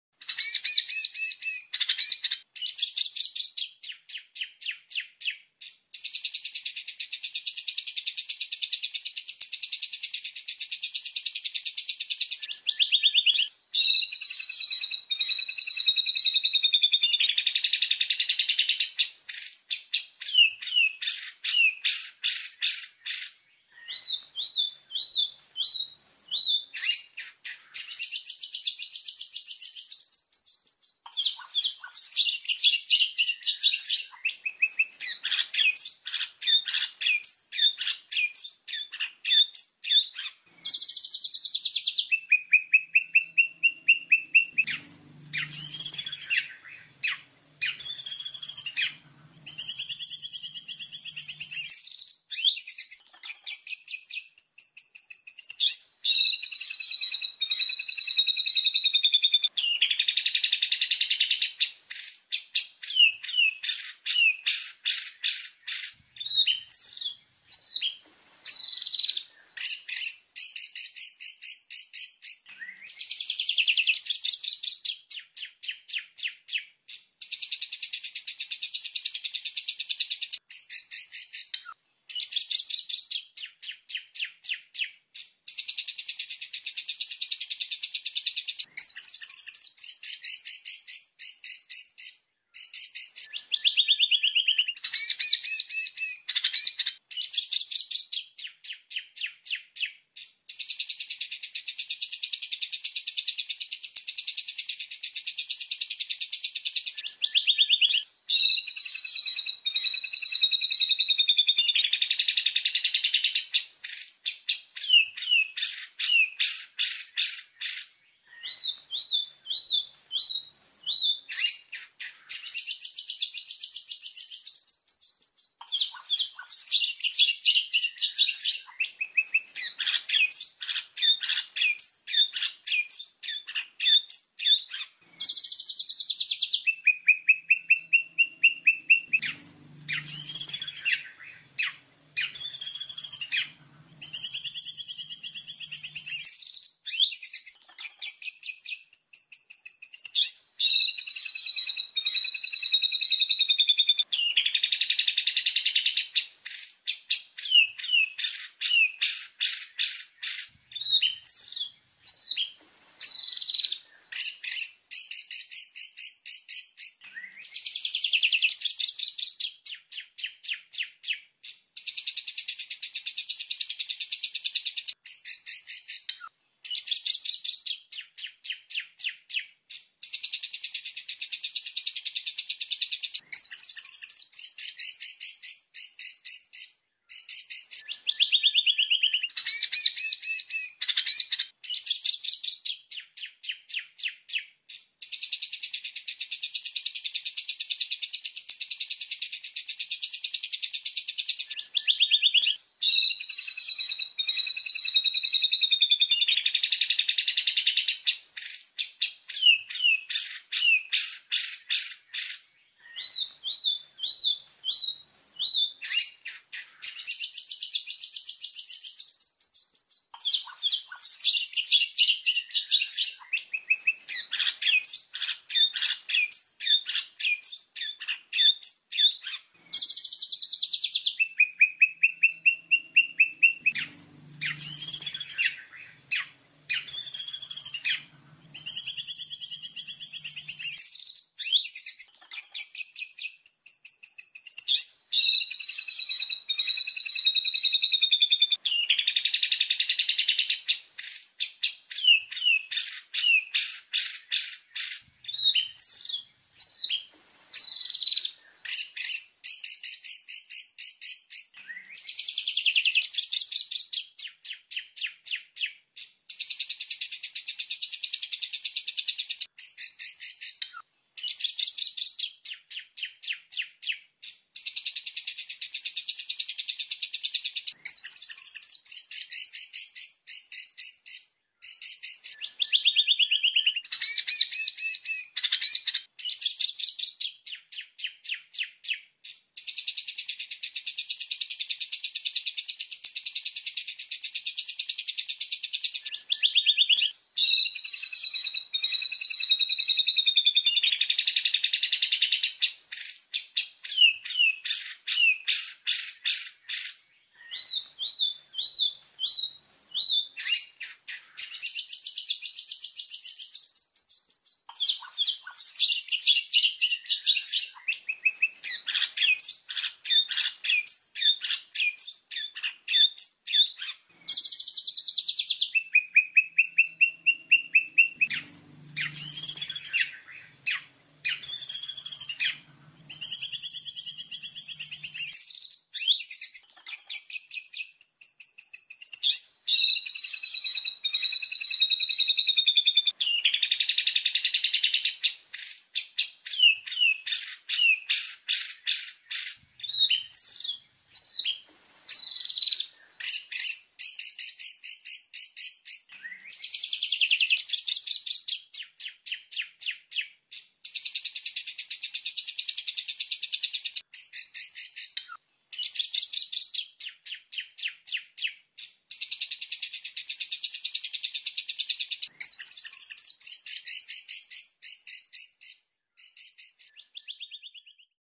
MASTERAN BURUNG LOMBA || MATERI TEMBAKAN KASAR UNTUK BURUNG LOMBA
Assalamualaikum,

Salam satu hobi Salam kicau mania , saya apload lagi suara burung masteran terbaru berikut ini adalah. MASTERAN BURUNG LOMBA || MATERI TEMBAKAN KASAR UNTUK BURUNG LOMBA. Suara burung tembakan kasar ini bagus untuk isian burung lomba khususnya burung kicau ,semoga bermanfaat. Terimakasih atas dukungannya , semoga bisa memberikan manfaat  untuk para kicau mania. Video ini di sajikan untuk para kicau mania untuk memenuhi kebutuhan yaitu masteran untuk burung peliharaan atau burung lomba

Wassalamu'alaikum.